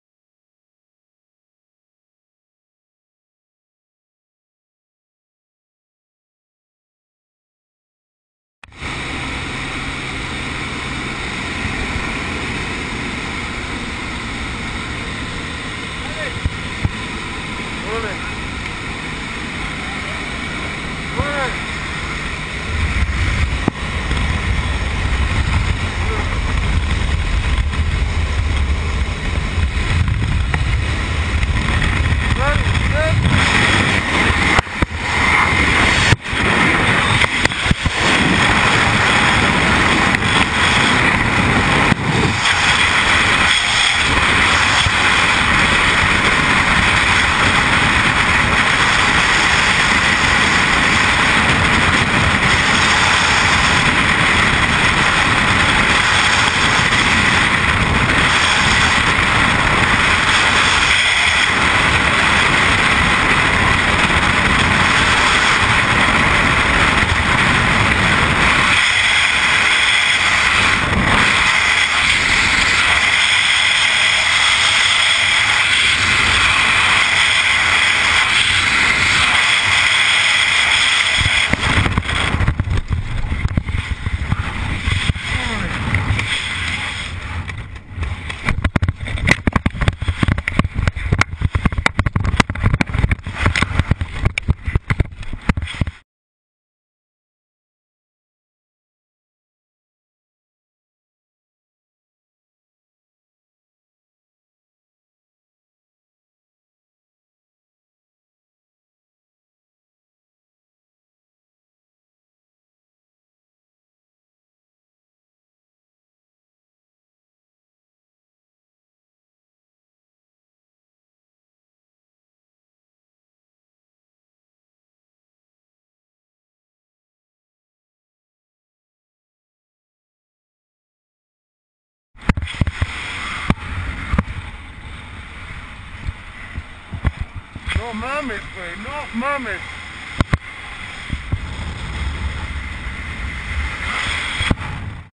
Near DEATH experience....Parachute Gets Tangled
Close encounter with death of the parachute jumper whos parachutes in tangled up thousands of feet above the ground.